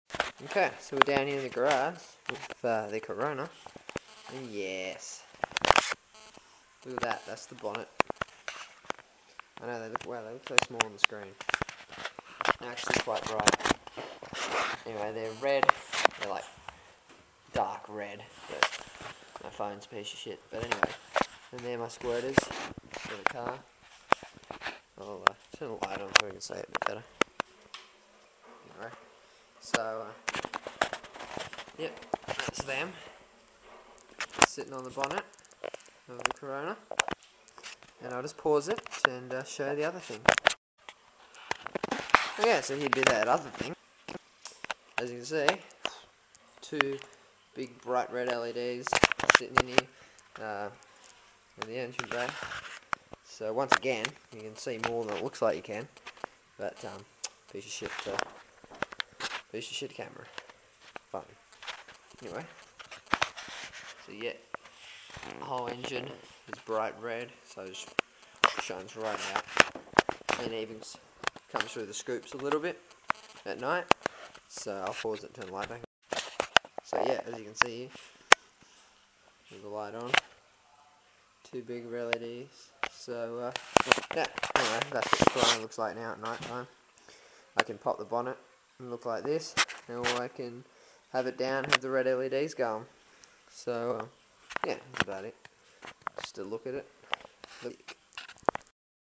Toyota Corona RED Engine Bay and Squirters
pretty G hay, took about 2 hour two install, squirters work great